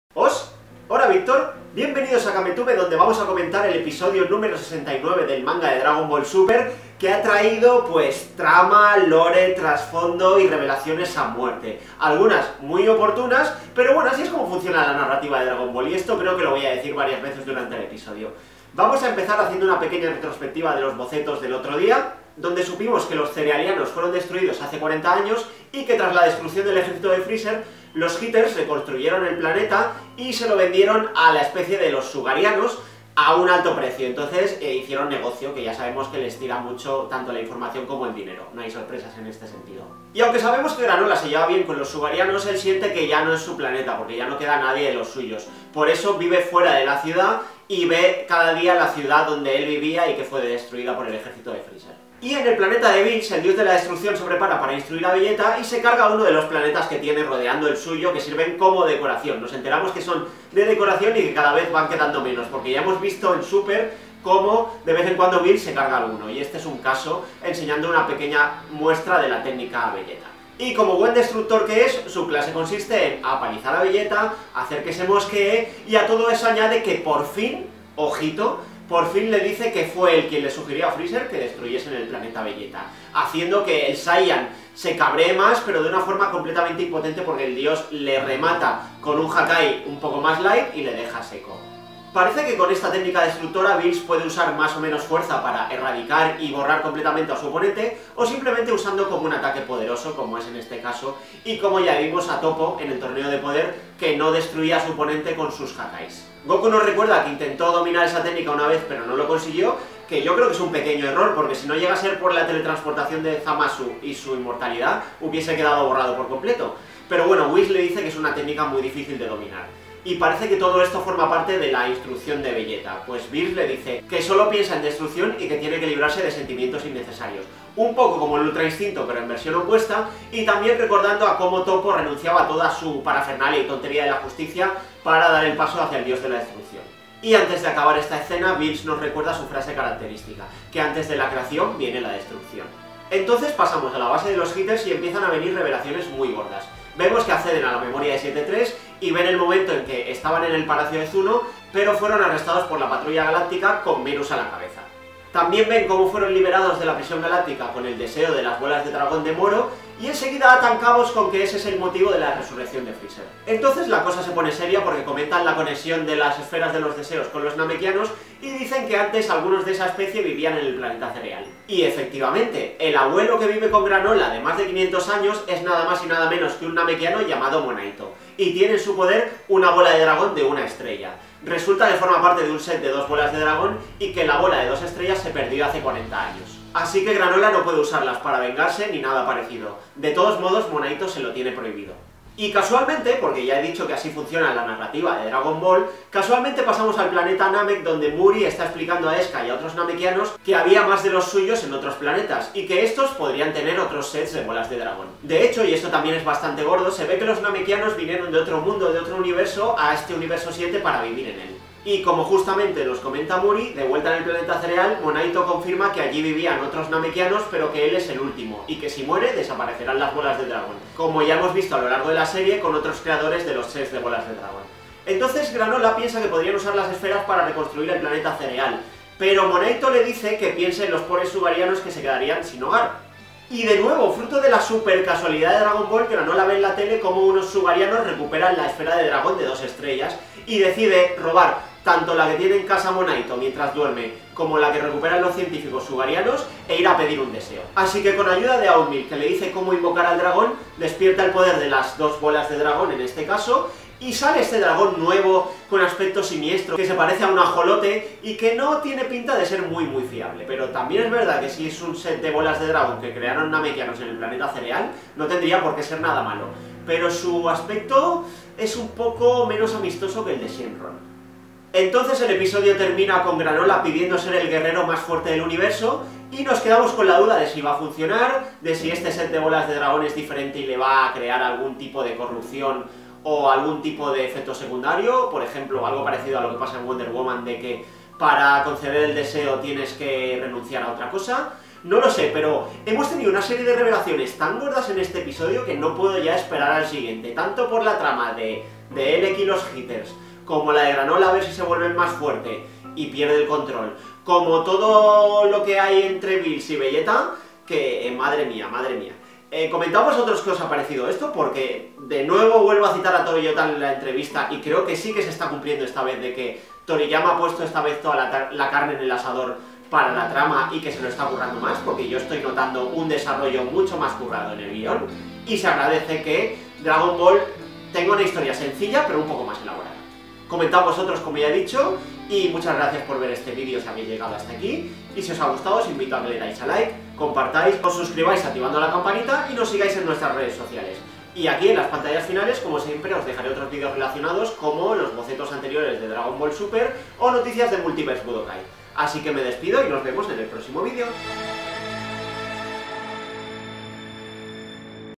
Dragon Ball SUPER MANGA 69 - Análisis, curiosidades y predicciones | Los caminos de Vegeta y Granola
Comentamos el manga 69 de Dragon Ball Super.